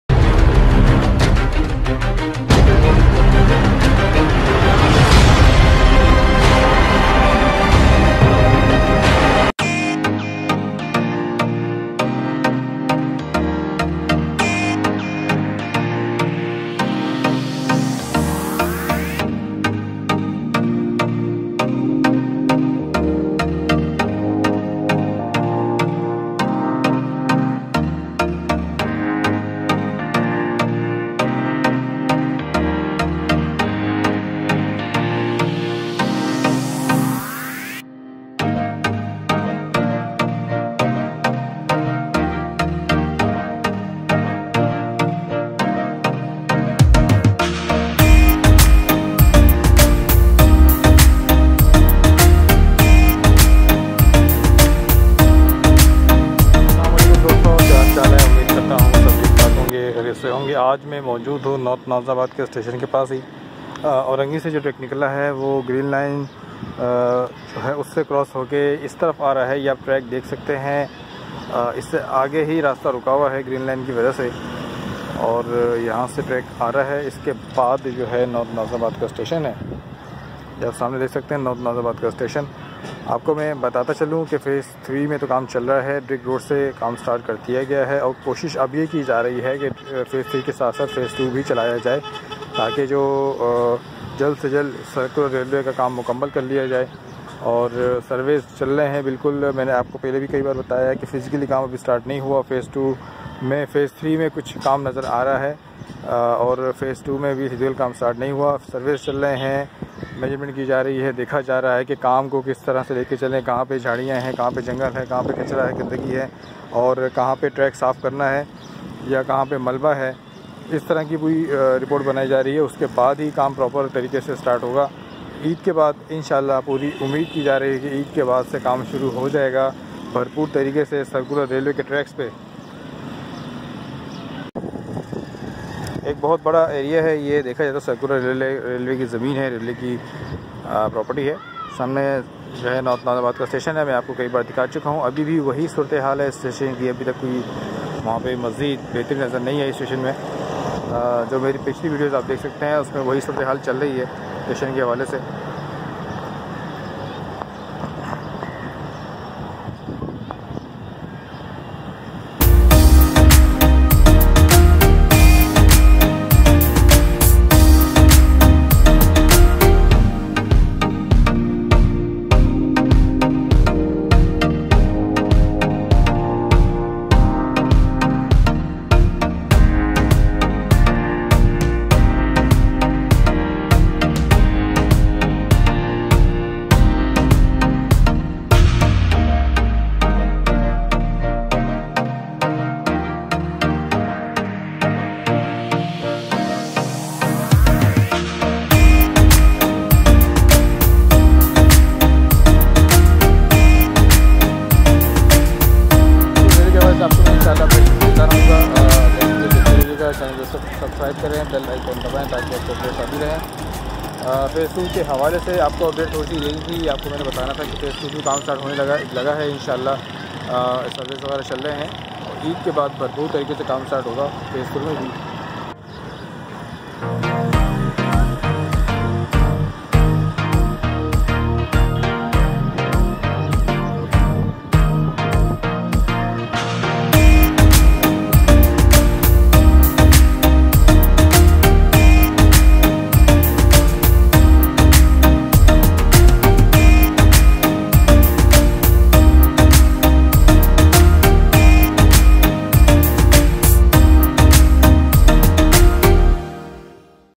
KCR Phase 2 Work Starts soon
Phase 2 main Kaam ab jald hi start hoga surveys mukammal or ab Kaam hota nazar ayega